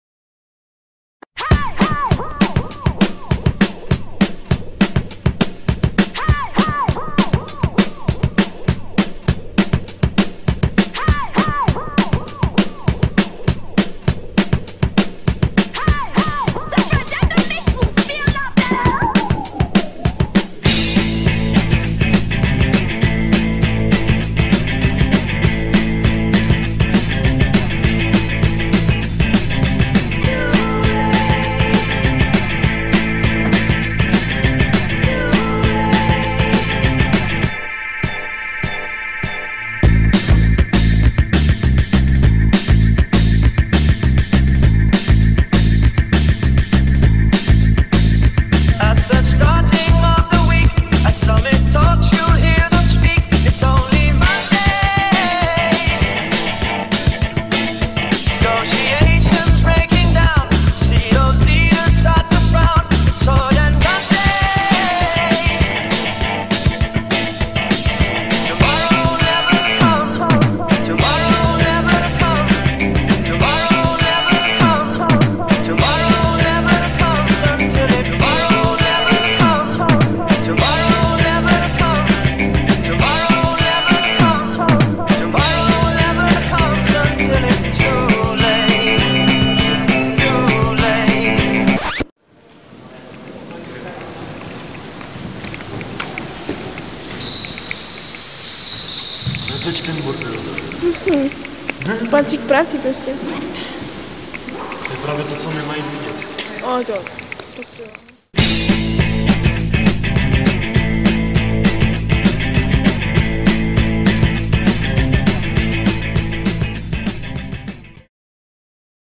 Hazená Sokol Karviná 18. 11. 2011 - focení
focení hráček jednotlivých družstvev na web, plakáty a tiskoviny